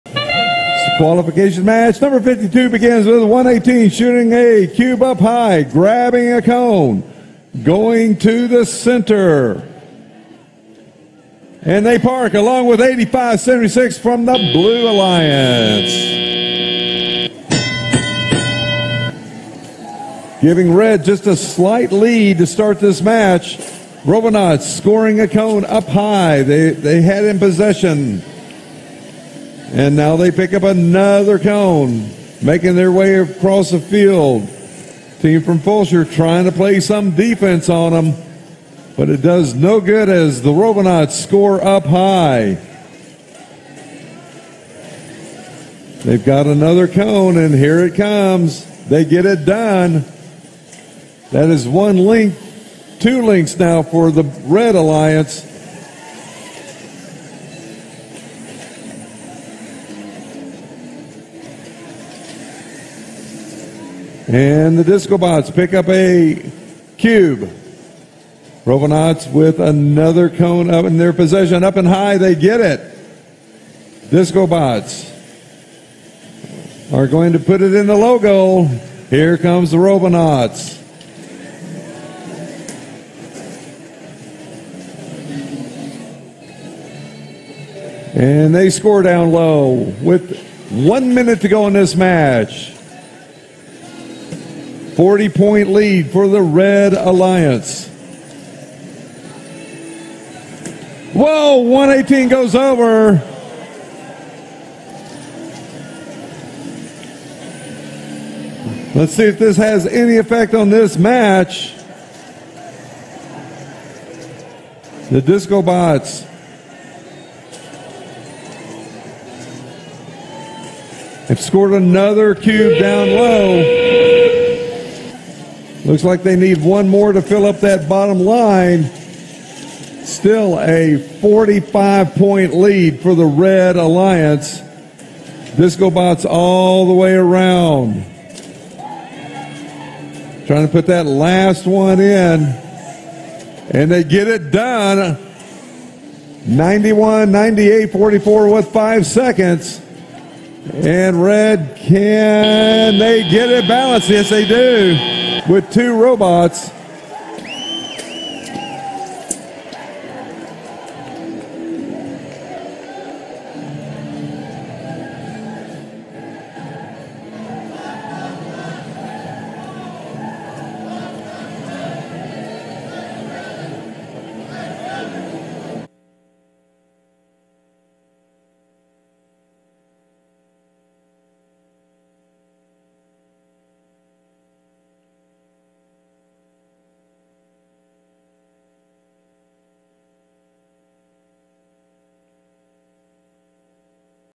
Houston 2023 Q52 - Robonauts Fall Over !
Houston 2023 Q52 - Robotics Competition
DiscoBots & Robonauts work together
Robonauts Goes sideways halfway through match.